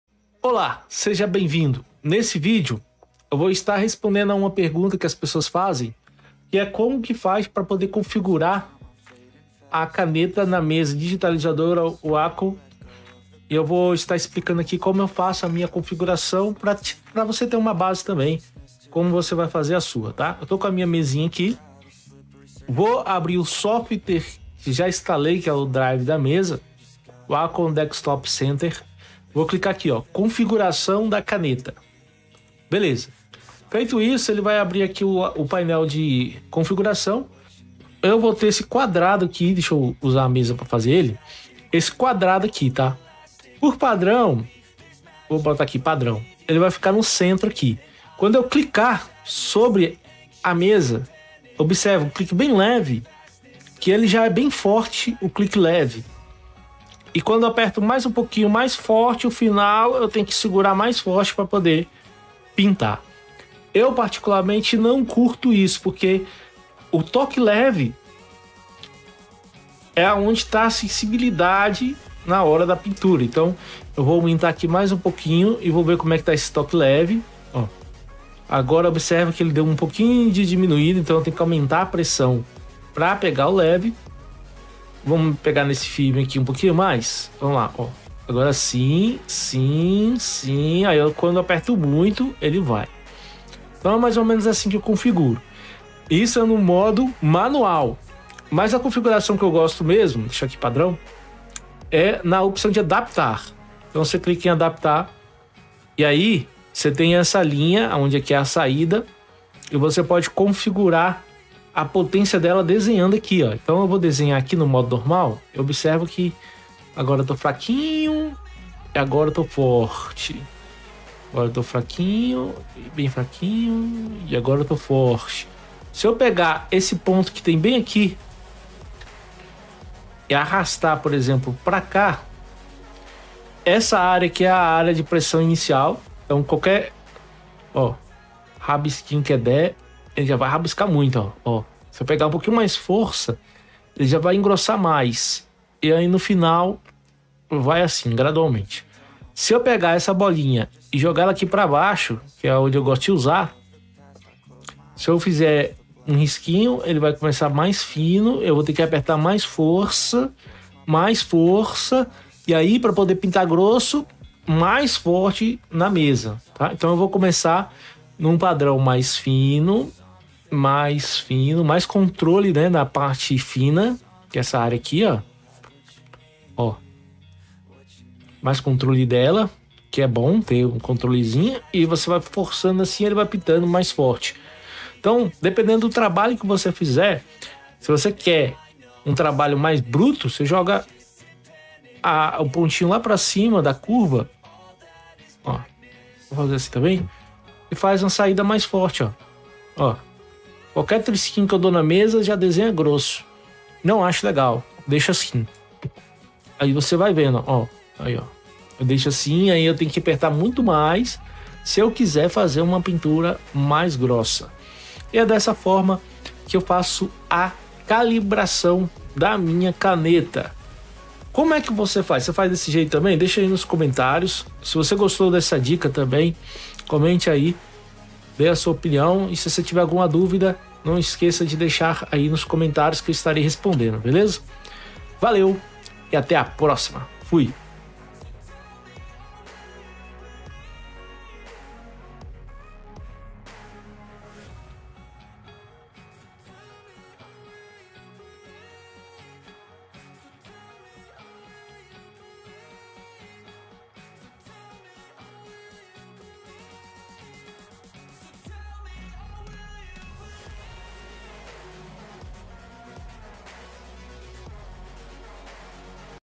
COMO CALIBRAR A CANETA WACOM
Se você comprou sua mesa digitalizadora Wacom após ter instalado o drive dela, deve ter perguntado para si. Como faço para calibrar a caneta corretamente.
Neste vídeo estarei mostrando a como fazer para calibrar corretamente sua mesa digitalizadora Wacom e entender melhor como funciona o modo de calibragem.

Se você tem alguma dúvida de como usar a mesa digitalizadora Wacom basta deixar nos comentários que estarei realizando um vídeo.
É muito importante calibrar a caneta da mesa digitalizadora para poder exercer a melhor precisão sobre seus projetos, neste vídeo falo simplificadamente de como calibrar corretamente a caneta Wacom para trabalhar no Photoshop ou em seu programa favorito.
Considero a mesa Wacom a melhor marca de mesa digitalizadora e até o modelo mais simples mostrado aqui poderá te ajudar em seu trabalho e claro, quando instalar o ‘drive’ da mesa poderá configurar para poder calibrar corretamente.
A vantagem de usar uma mesa digitalizadora é poder realizar um trabalho mais preciso e efetuar um melhor acabamento em suas edições.
Então vamos que vamos.

►Ajude o canal com 1 real.